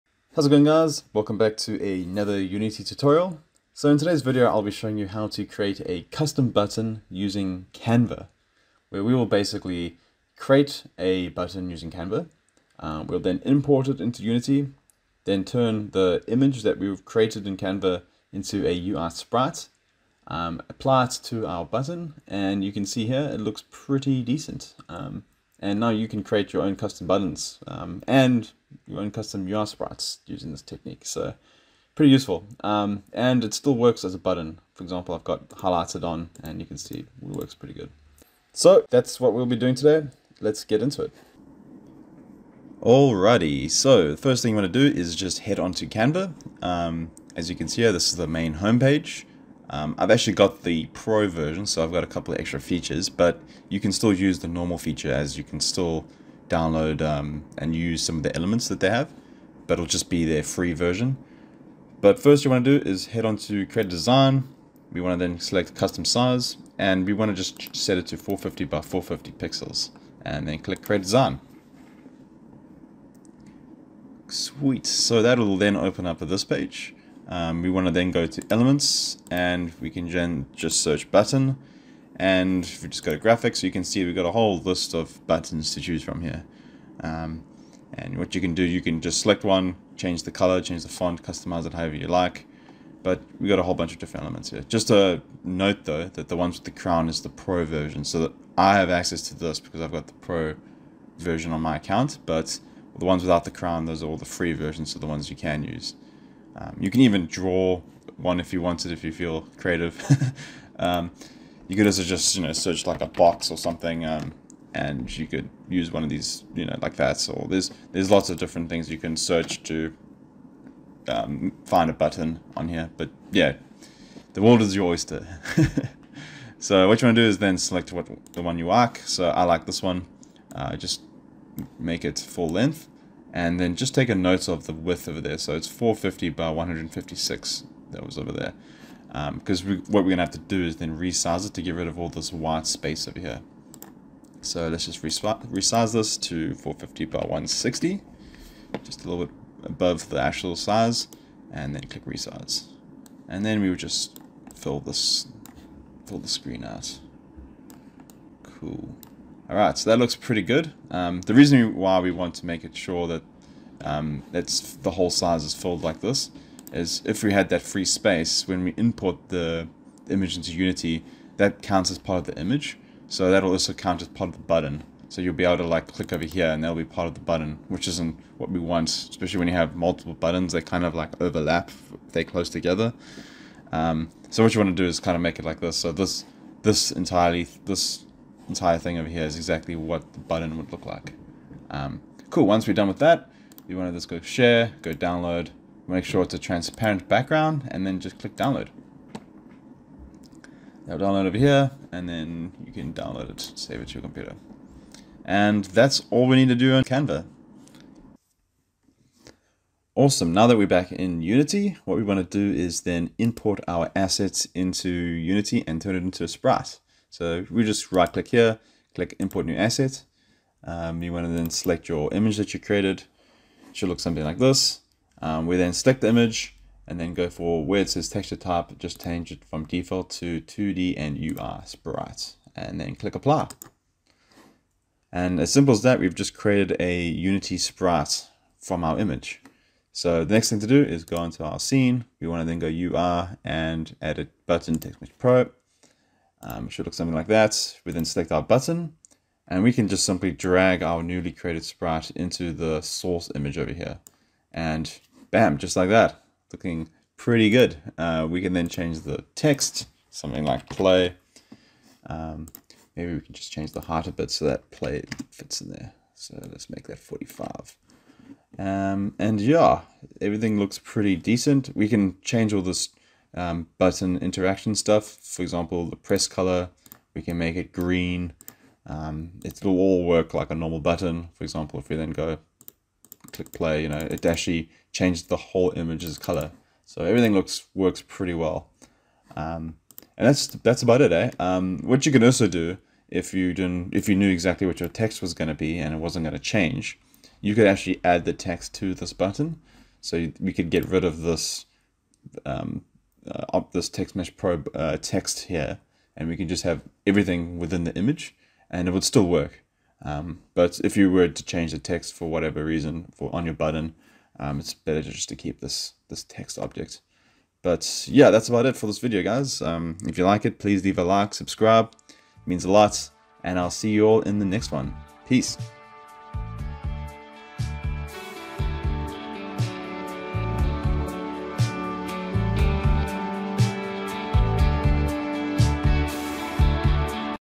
Unity Tutorial : How to create a custom button using Canva ( Main Menu or in game buttons )
A simple tutorial on how to create your own buttons or UI Sprits using Canva for your main menu or different game aspects.